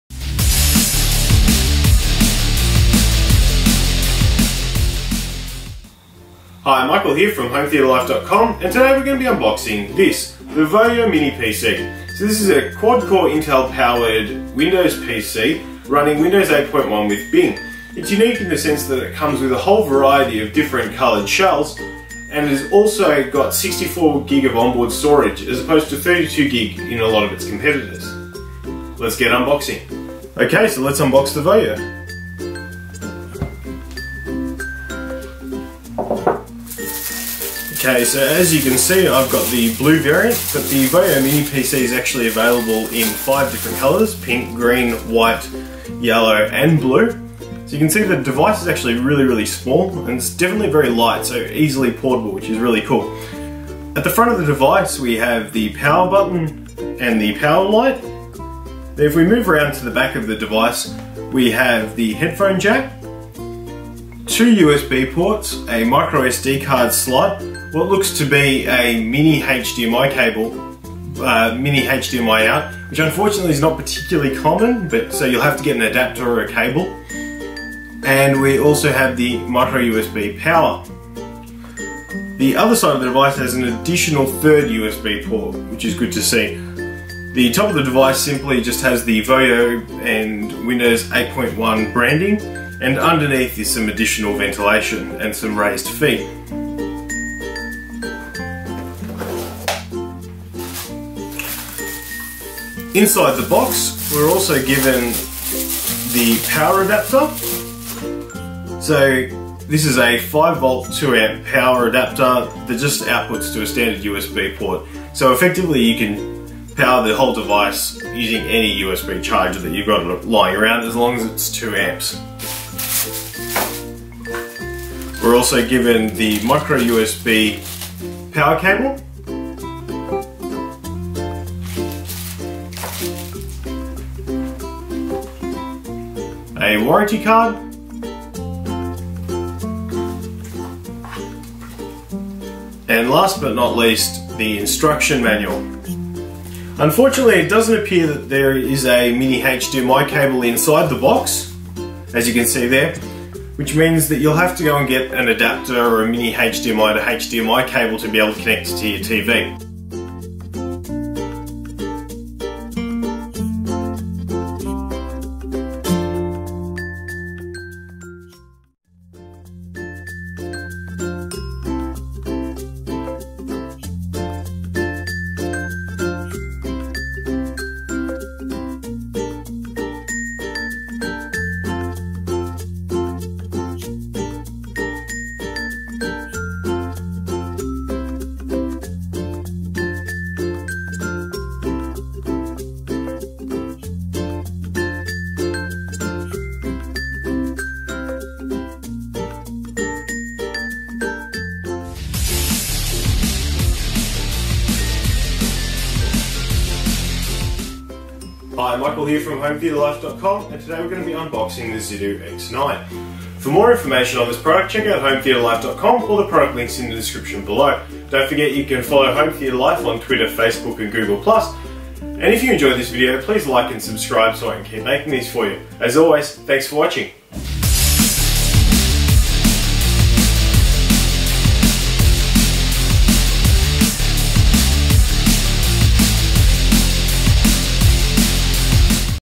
VOYO Mini PC Unboxing and Demo: Quadcore Intel Mini PC running Windows 8.1
Review The VOYO Mini PC is an colorful quadcore Windows Mini PC for under $130.

Technical Specs
Chipset: Intel Baytrail quad core processor with Intel HD graphics (Z3735F)
RAM: 2 GB DDR3
Storage: 64GB eMMC + microSD slot up to 128GB
Video & Audio Output: HDMI 1.4, 3.5mm Audio
Connectivity: 802.11 b/g/n Wi-Fi, Bluetooth 4.0
USB: 3x USB 2.0 port
OS: Windows 8.1 with Bing (32-bit)
Dimensions: 126 x 126 x 12mm
Weight: 140g